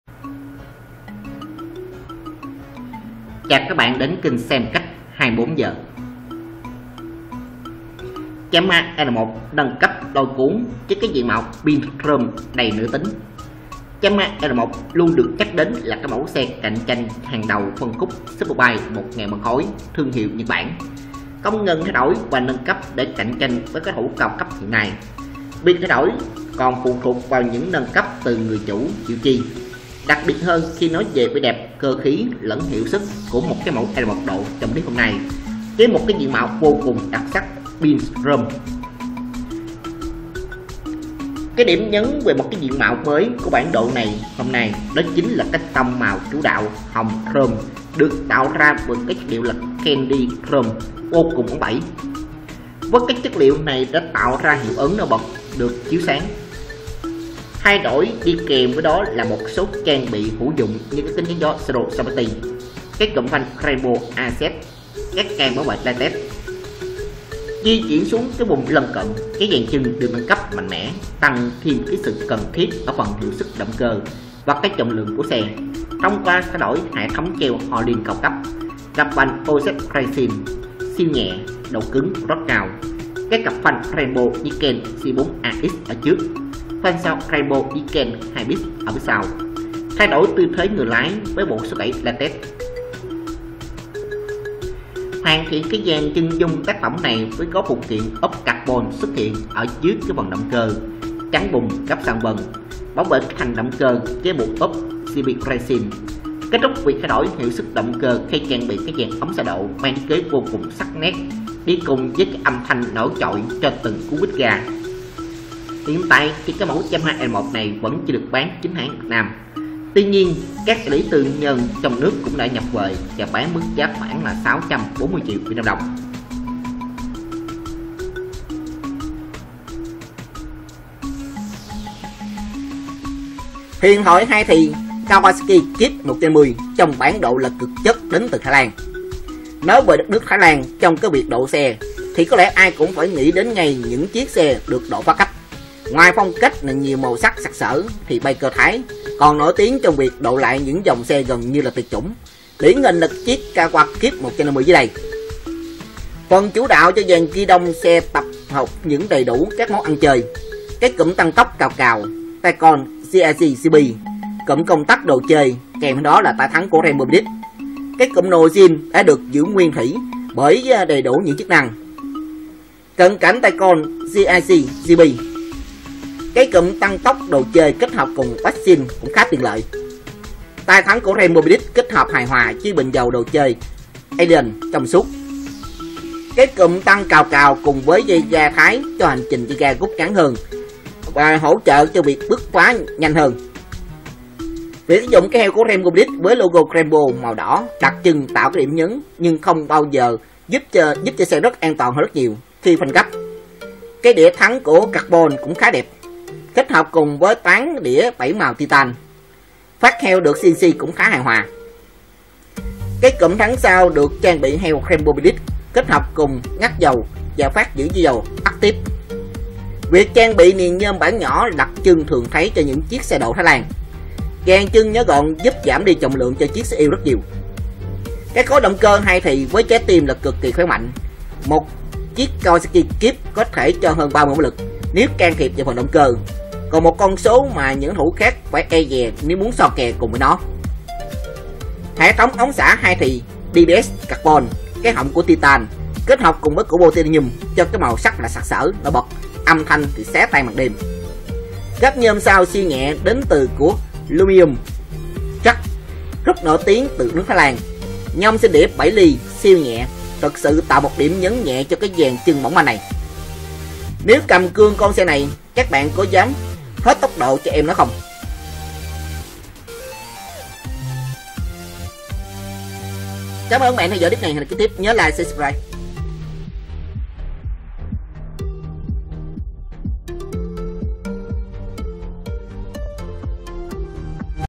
Xe & Phong Cách 24h - Yamaha R1 nâng cấp lôi cuốn với diện mạo Pink Chrome.
Yamaha R1 luôn được nhắc đến là mẫu xe cạnh tranh hàng đầu trong phân khúc Superbike 1.000 phân khối của thương hiệu Nhật Bản, không ngừng thay đổi và nâng cấp để cạnh tranh với các đối thủ cao cấp hiện nay, việc thay đổi đó còn phụ thuộc vào những nâng cấp từ các chủ xe chịu chơi. Đặc biệt hơn khi nói về vẻ đẹp cơ khí lẫn hiệu suất tuyệt vời của mẫu R1 độ trong clip hôm nay với diện mạo vô cùng đặc sắc "Pink Chrome".